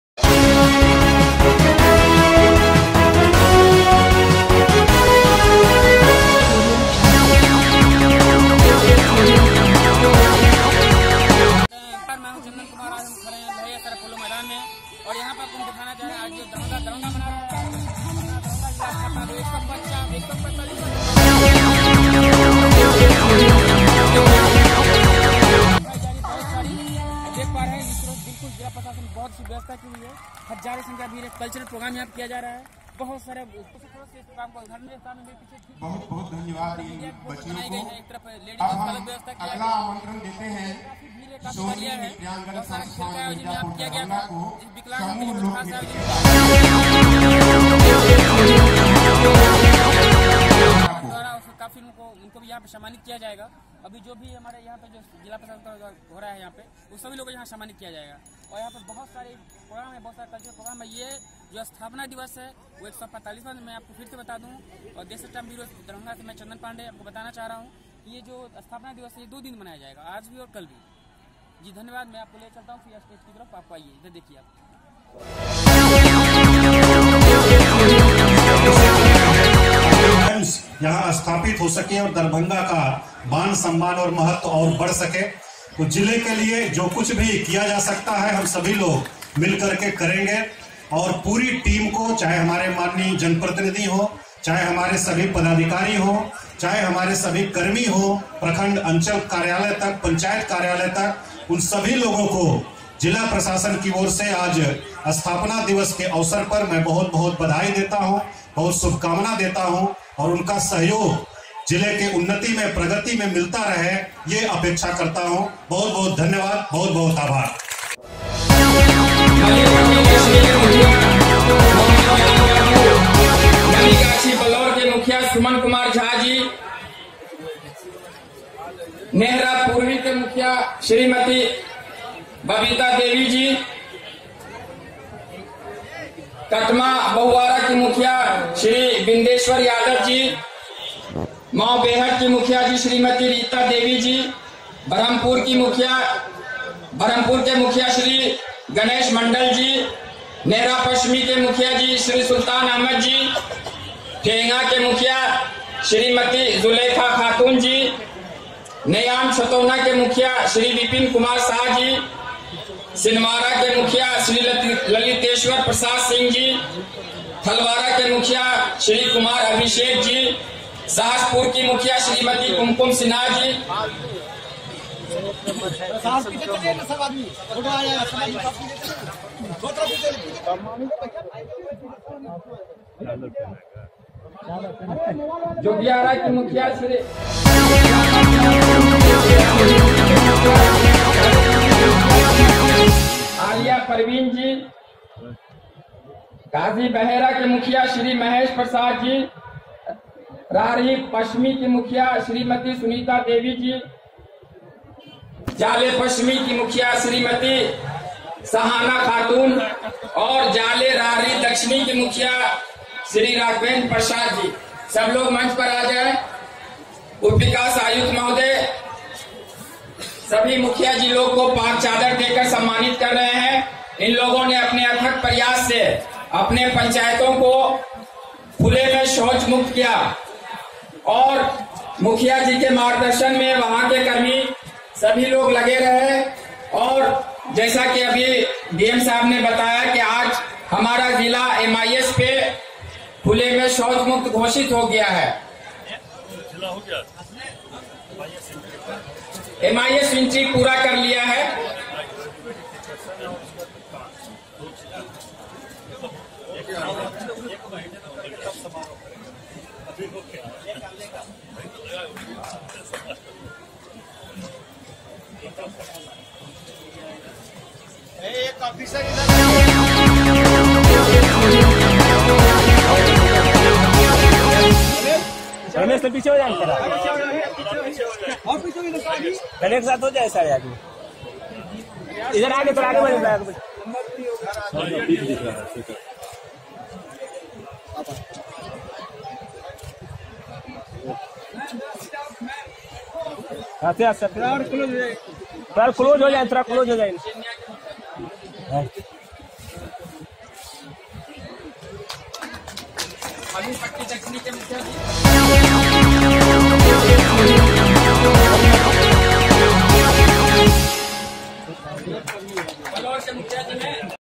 HappyBirthdayDarbhanga
145 साल का हुआ अपना दरभंगा।
नेहरू स्टेडियम में जिला स्थापना दिवस देशज टाइम्स के लिए चंदन पांडेय की रिपोर्ट